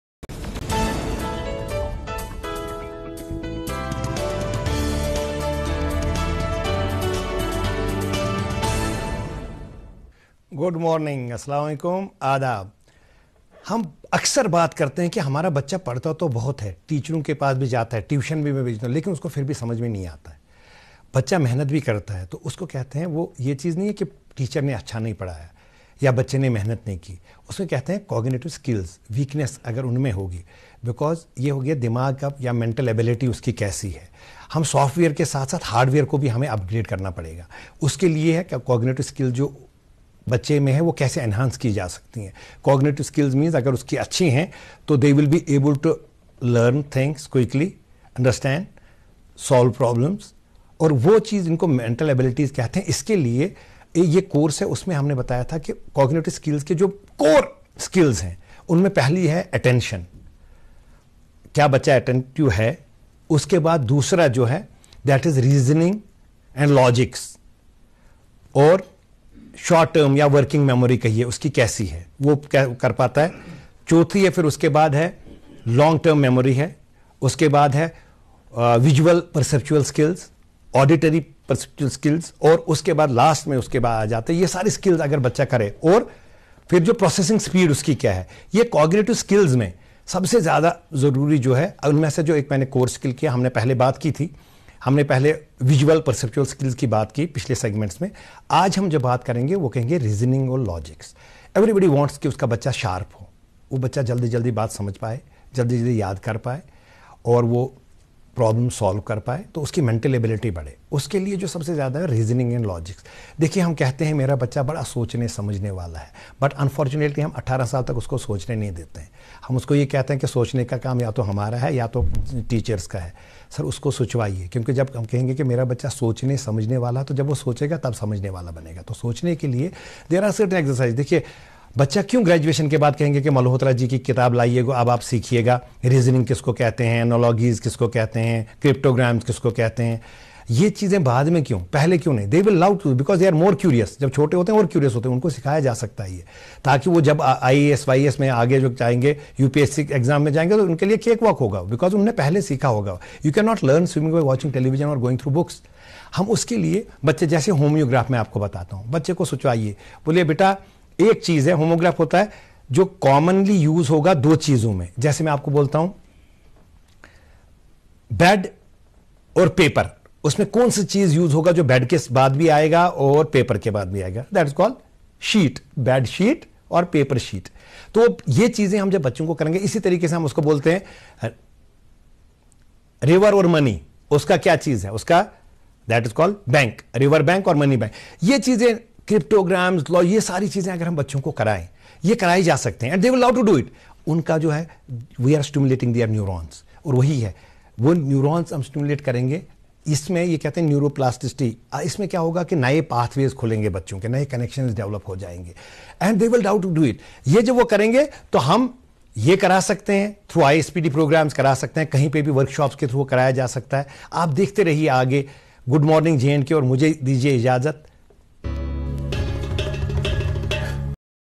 63 Seg. Importance of Developing Logical Reasoning, Cognitive Ability, and Critical Thinking Skills.
Logical reasoning, in combination with other cognitive skills, is an important skill you use during all kinds of daily situations. It helps you make important decisions, discern the truth, solve problems, come up with new ideas and set achievable goals.For thriving in this 21st-century world, one thing is crystal clear- to develop and prepare today's children for skills that go beyond the grading system.